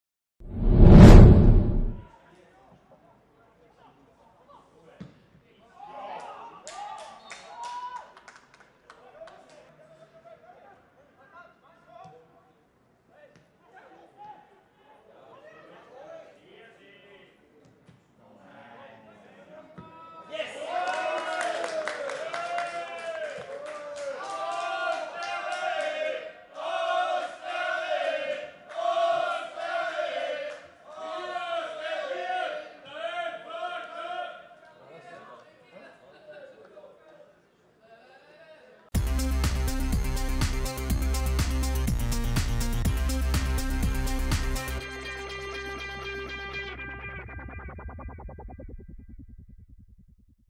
Tore des 1:1 gegen Slovan Liberec
Stefan Stangl trifft beim Testspiel des FK Austria Wien gegen Slovan Liberec auf Zypern.